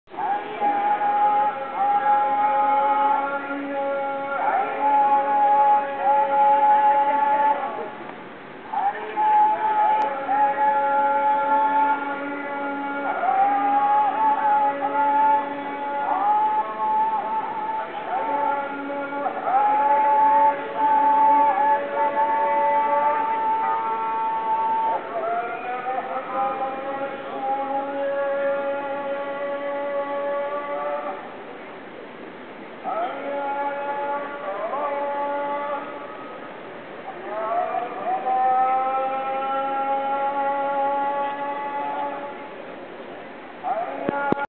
Plaza, Tanger, call to prayer
In Tanger, kicking it in a plaza, with my camera on my knee, the call to prayer